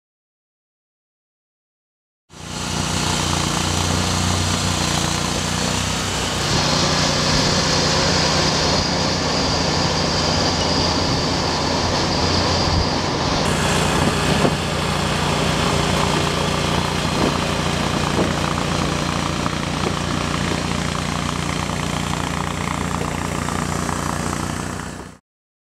Dixie Chopper's Fastest Propane Mower (HD)
Having some fun with a Dixie Chopper Propane Mower.  This is a modified version of our Xcaliber Propane machine.  There's a good reason why they call Dixie Chopper "The World's Fastest Lawn Mower."**

**For demonstration purposes only.  Roads were closed for safety.  Do not EVER attempt to drive any Dixie Chopper on roads or at high speeds.